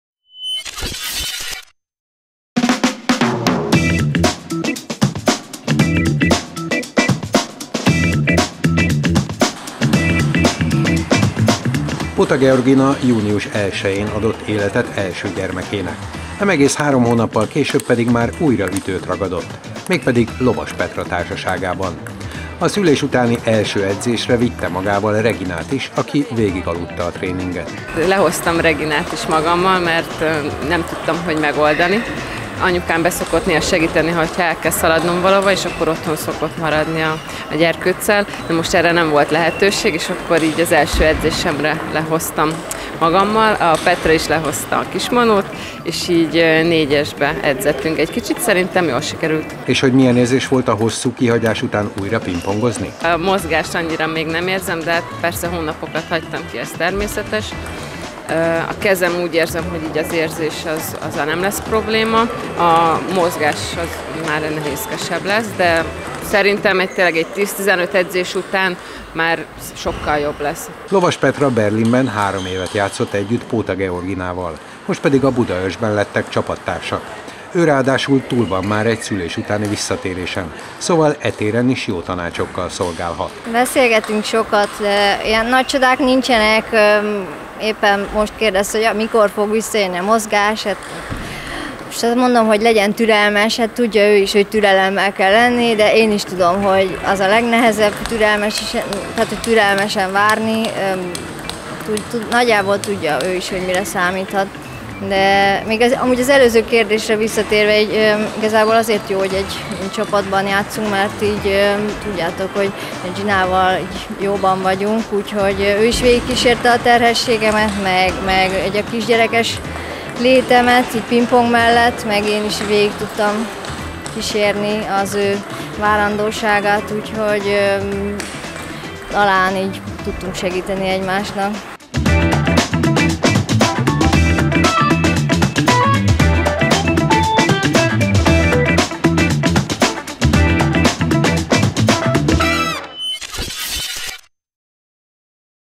MOATSZ Híradó 2020/6 ► Újra edz Póta Gina
Póta Georgina június 1-jén adott életet első gyermekének, nem egész három hónappal később pedig már újra ütőt ragadott, mégpedig Lovas Petra társaságában. A szülés utáni első edzésére vitte magával Reginát is, aki végigaludta a tréninget.
Interjúk: Póta Georgina, Petra Lovas
Minden jog fenntartva: Magyar Asztalitenisz Szövetség